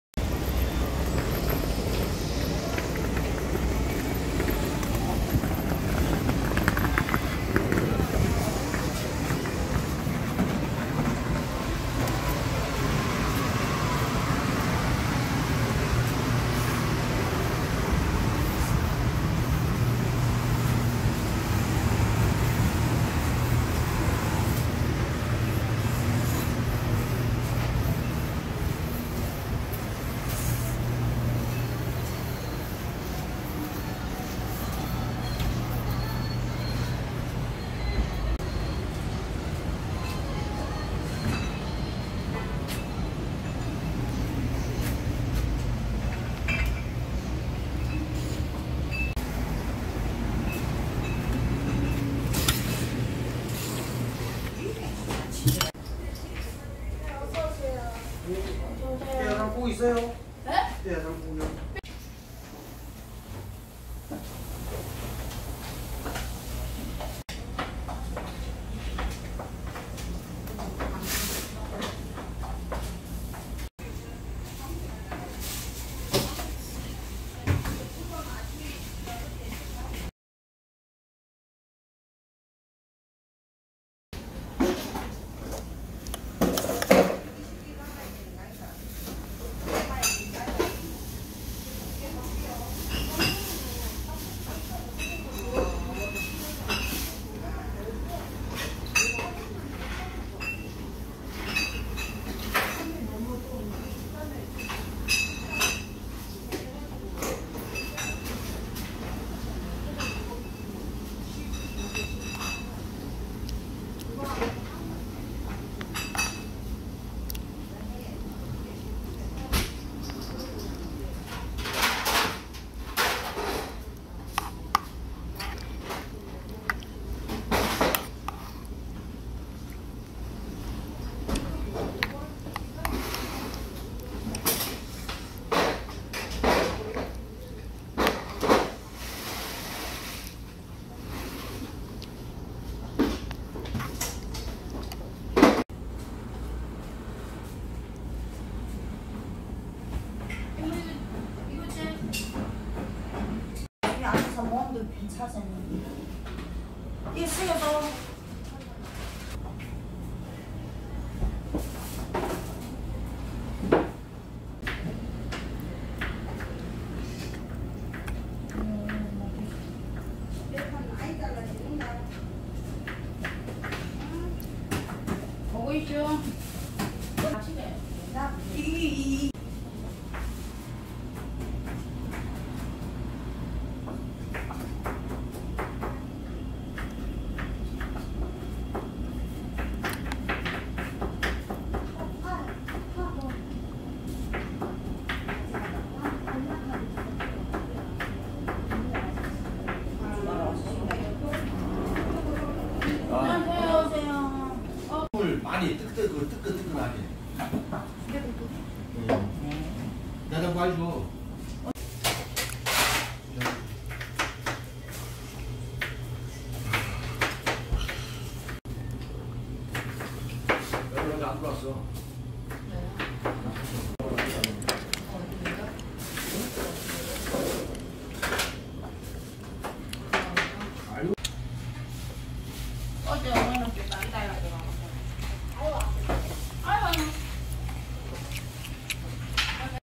[경기 수원](수원역 순대국골목) 아다미순대국 순대국&모듬수육
수원역 순대국골목에 위치한 아다미순대국에서 순대국과 모듬수육으로 점심식사를 했습니다.
참고로 순대국은 온도가 적당해서 입천장을 데이지 않을 정도입니다.